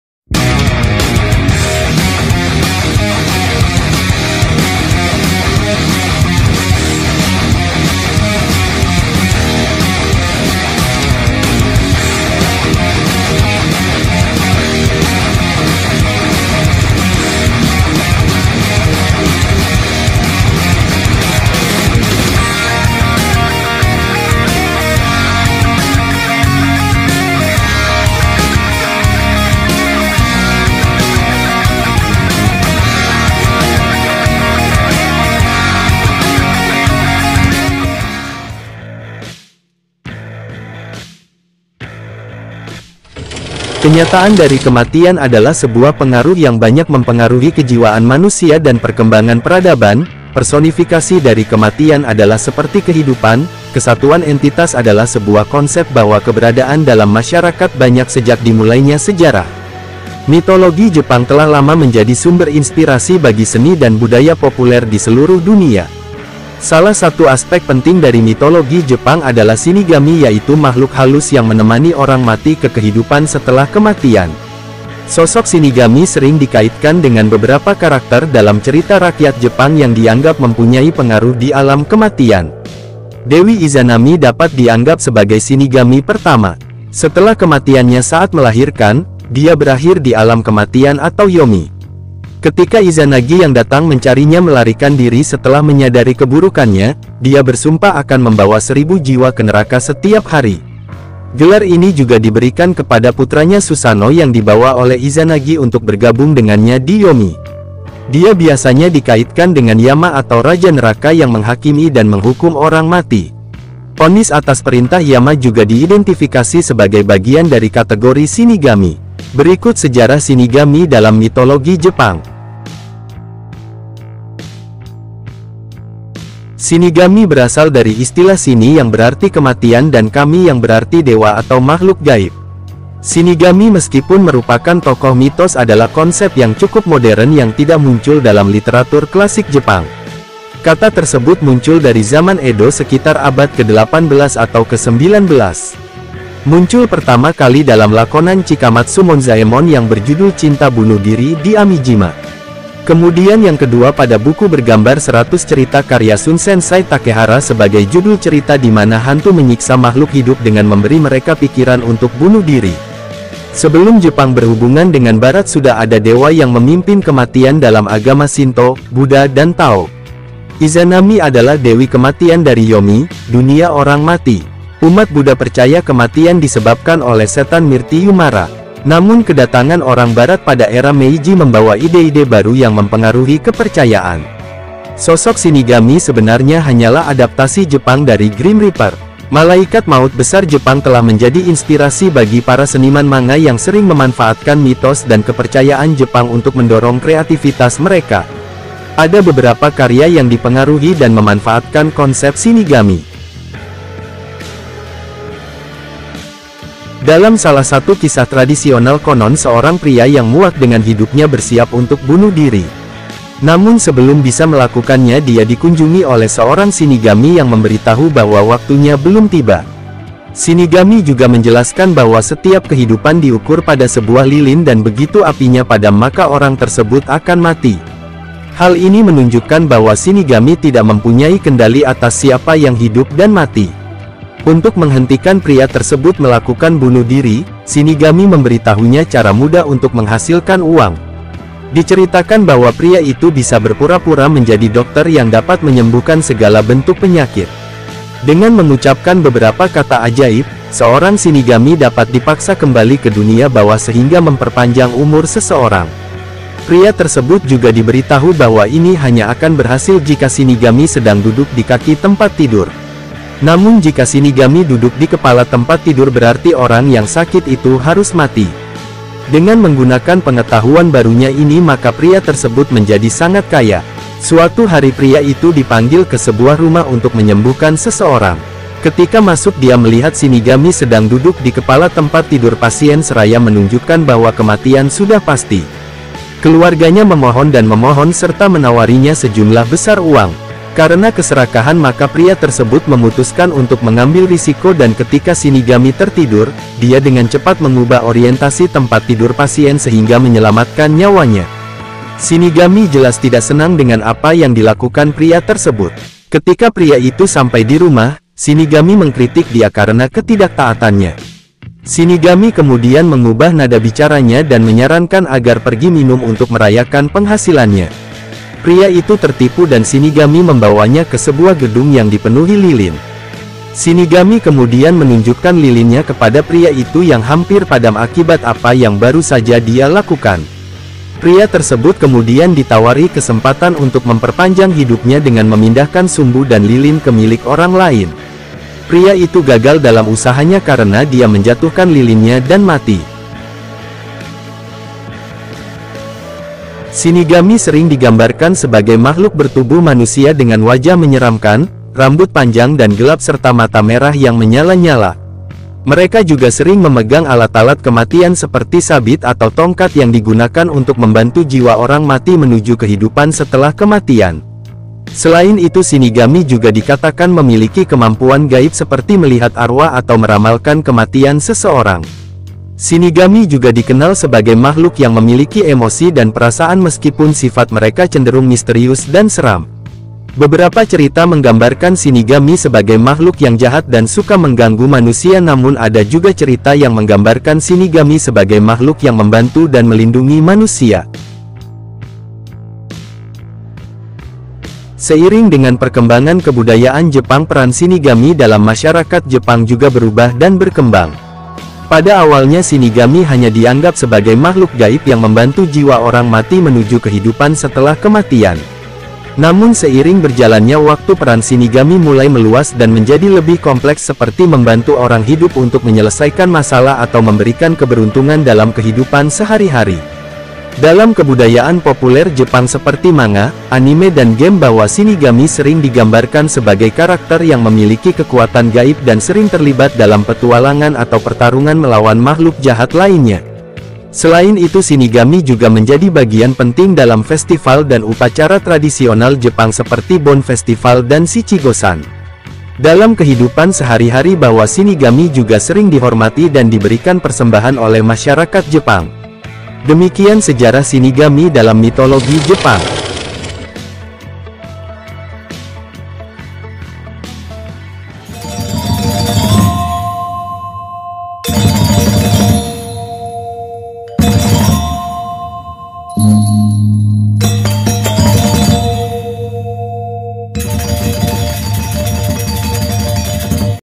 Mitologi Shinigami (死神) Tentang Dewa Kematian Jepang
Sosok Shinigami sering dikaitkan dengan beberapa karakter dalam cerita rakyat Jepang yang dianggap mempunyai pengaruh di alam kematian.